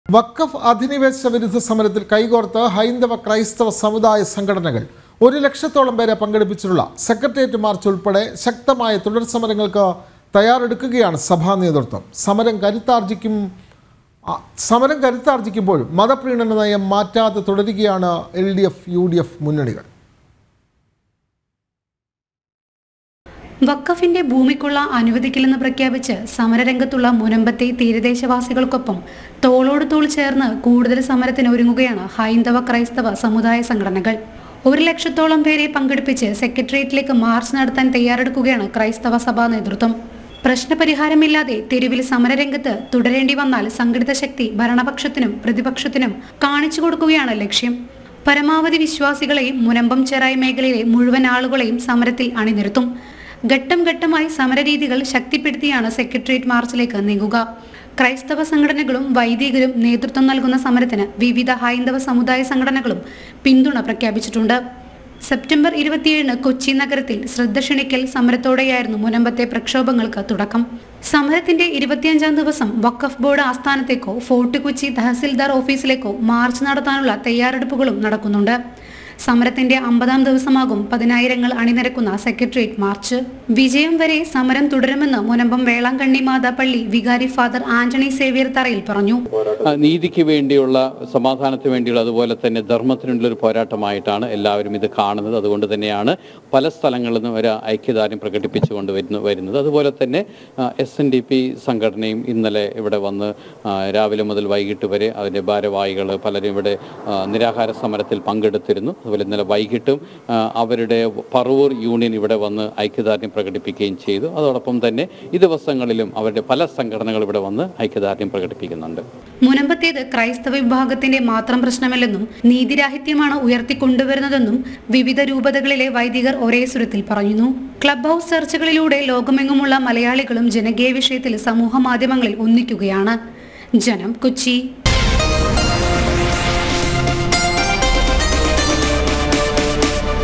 "വഖഫ് കയ്യേറ്റ വിരുദ്ധ സമരത്തിൽ കൈകോർത്ത് ഹൈന്ദ-ക്രൈസ്തവ സമുദായ സംഘടനകൾ" | WAQF
വഖഫ് അധിനിവേശ വിരുദ്ധ സമരത്തിൽ കൈകോർത്ത് ഹൈന്ദ-ക്രൈസ്തവ സമുദായ സംഘടനകൾ. ഒരു ലക്ഷത്തോളം പേരെ പങ്കെടുപ്പിച്ചുള്ള സെക്രട്ടറിയേറ്‍റ് മാർച്ച് ഉൾപ്പെടെ ശക്തമായ തുടർ സമരങ്ങൾക്ക് തയ്യാറെടുത്ത് സഭാ നേതൃത്വം. സമരം കരുത്താർജ്ജിക്കുമ്പോഴും മത പ്രീണന നയം മാറ്‍റാതെ LDF, UDF മുന്നണികൾ.